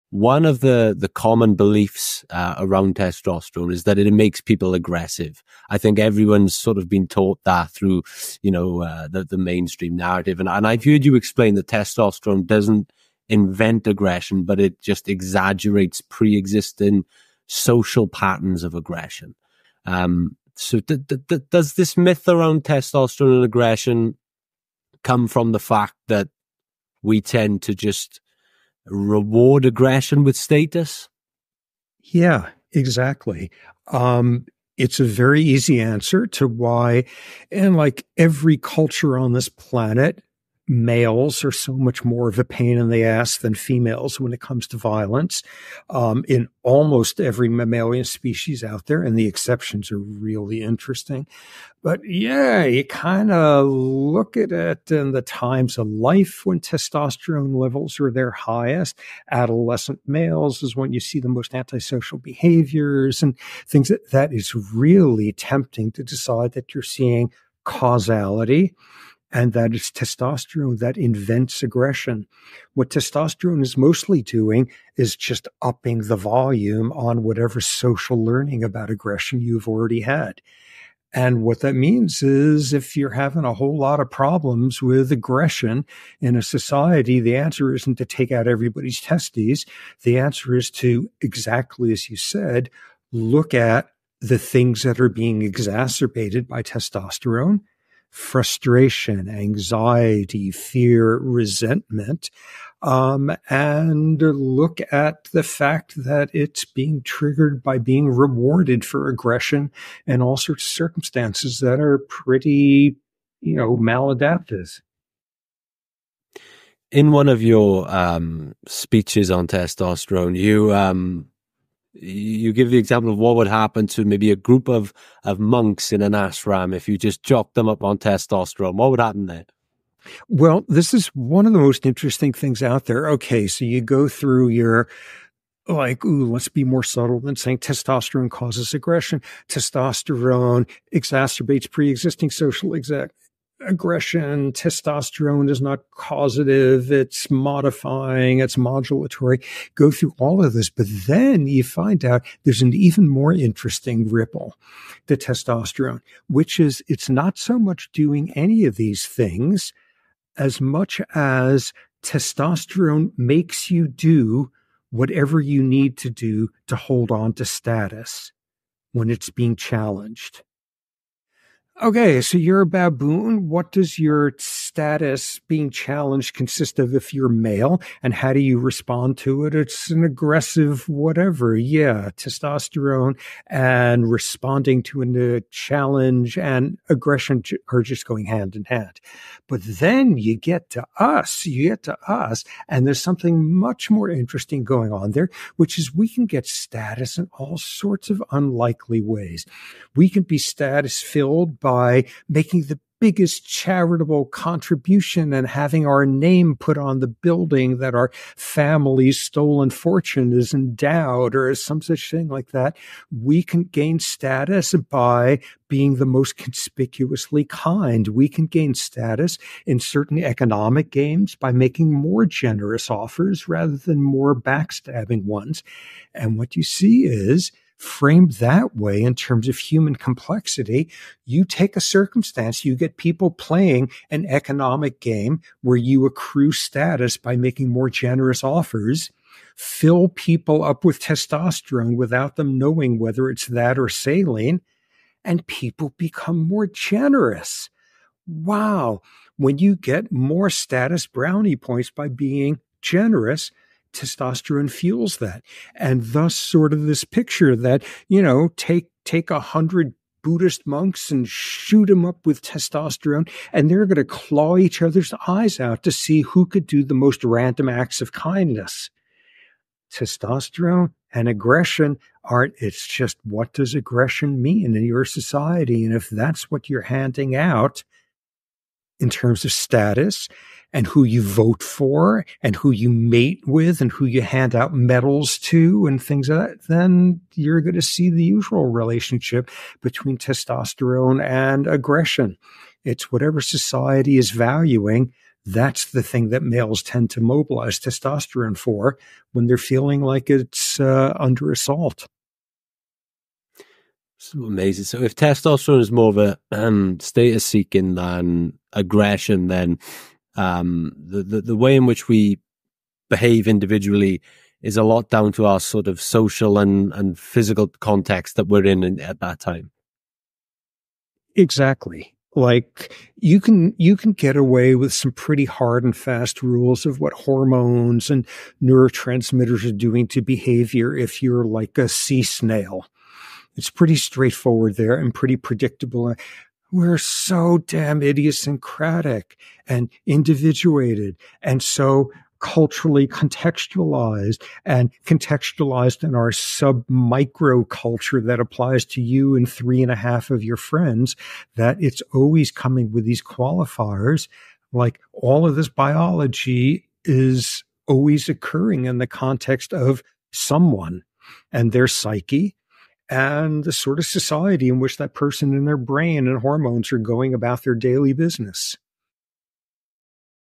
Nobody Knows THIS About Testosterone | Robert Sapolsky
Dr Robert Sapolsky explains how testosterone amplifies behaviours that gain status, and what that means if you boosted the testosterone of a group of Buddhist monks.